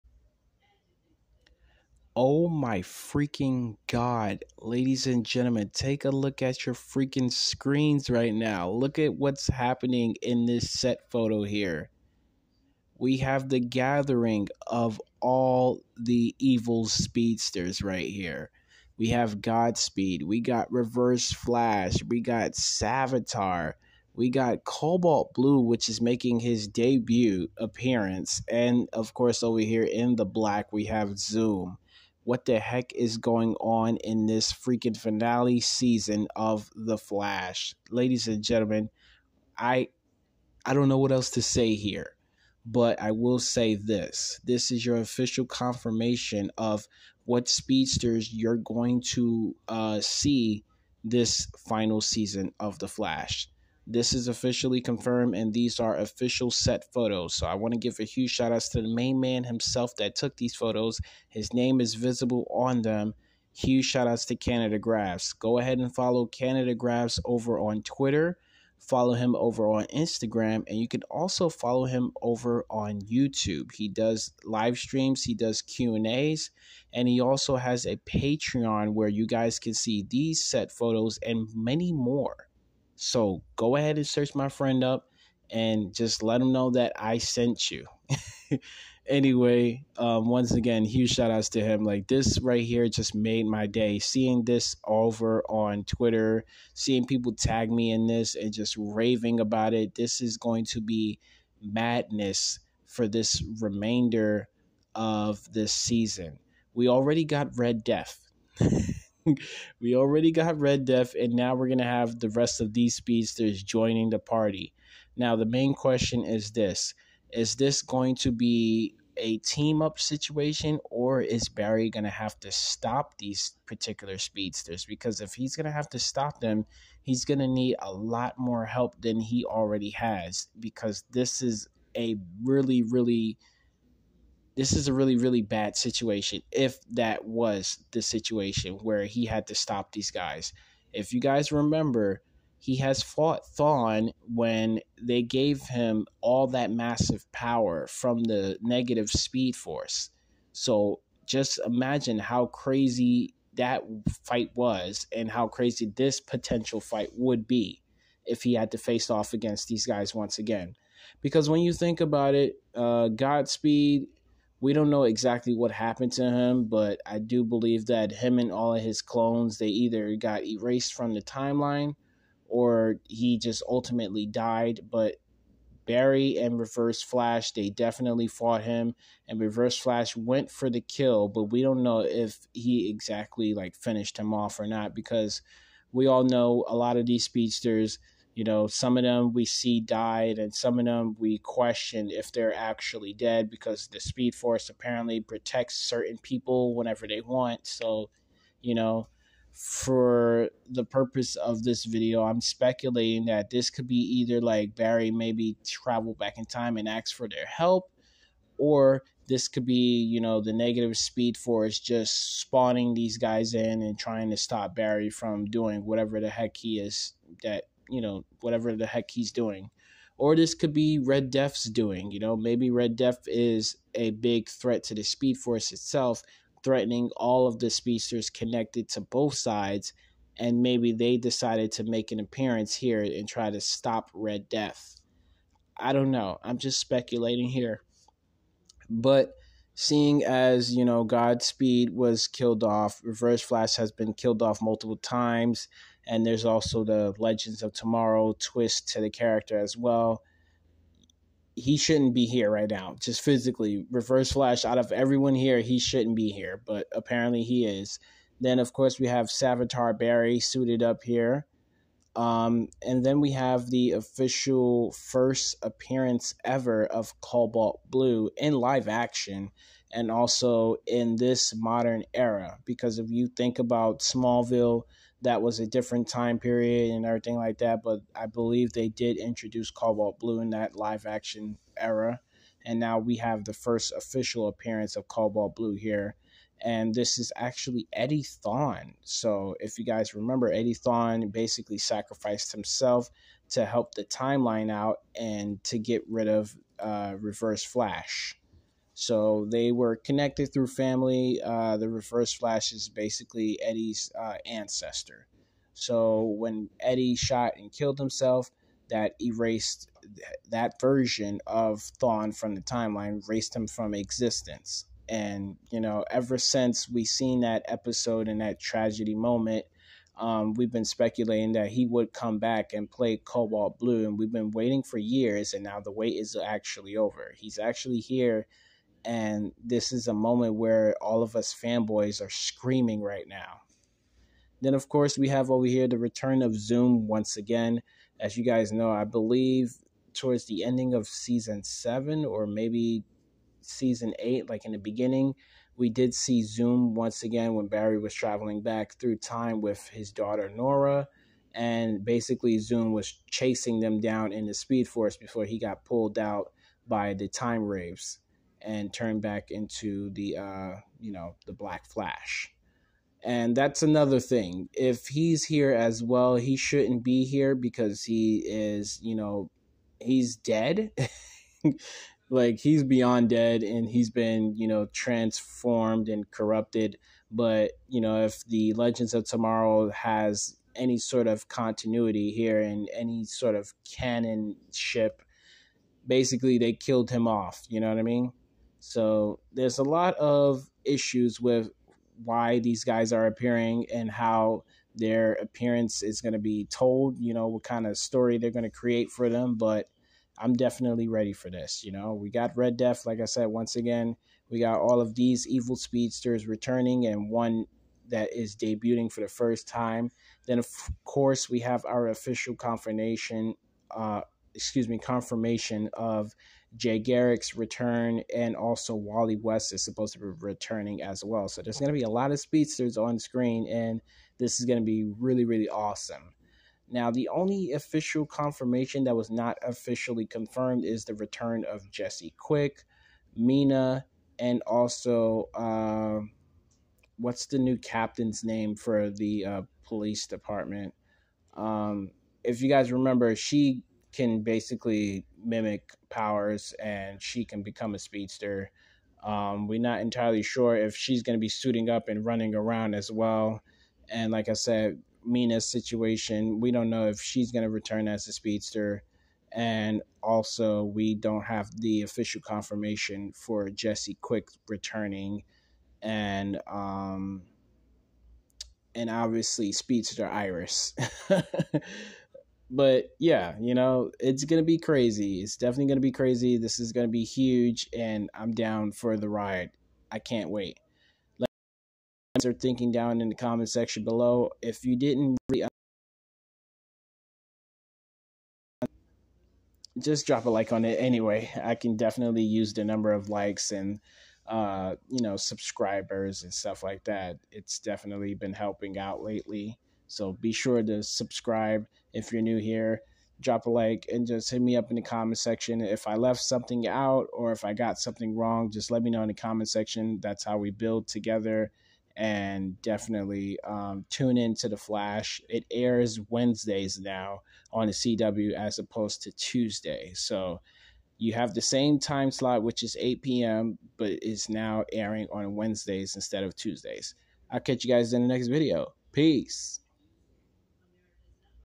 All Villainous Speedster Return with the debut of Cobalt Blue
What could possibly be happening with this gathering of all the bad speedsters? The only bad guy missing I would say is The Rival

You are the best there is man @Canadagraphs
Guys make sure to check him out on all platforms